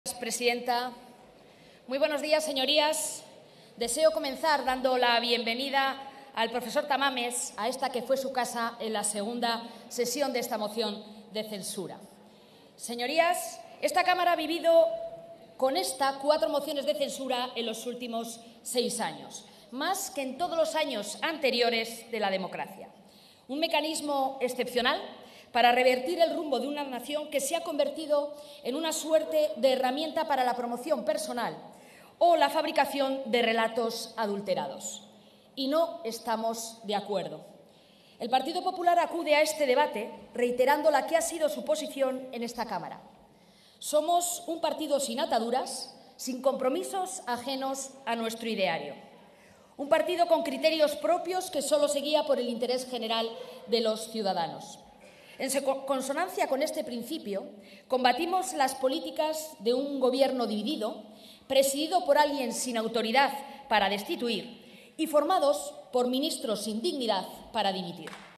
PP cree que la moción convierte a Yolanda Díaz en la marca blanca de Sánchez
La portavoz del Grupo Popular en el Congreso, Cuca Gamarra, ha asegurado hoy que las mociones de censura, como la presentada por Vox y que se discute desde ayer en el Parlamento, se han usado para la promoción personal. En su opinión, la intervención de ayer de la vicepresidenta segunda del Gobierno, Yolanda Díaz, ha demostrado que es la "marca blanca" de Pedro Sánchez en Podemos.
(Fuente: Congreso)